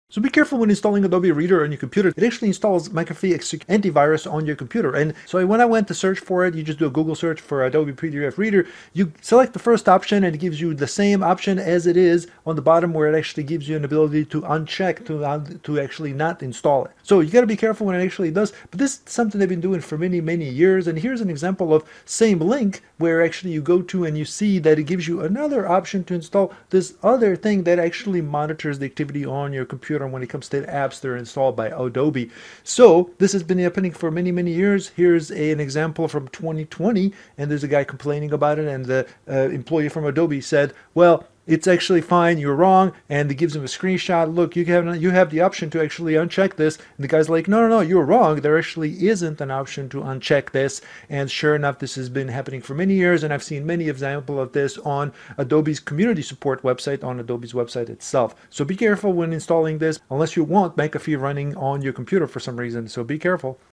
Warning Adobe Reader installs McAFee bloatware and AGS on your computer
My equipment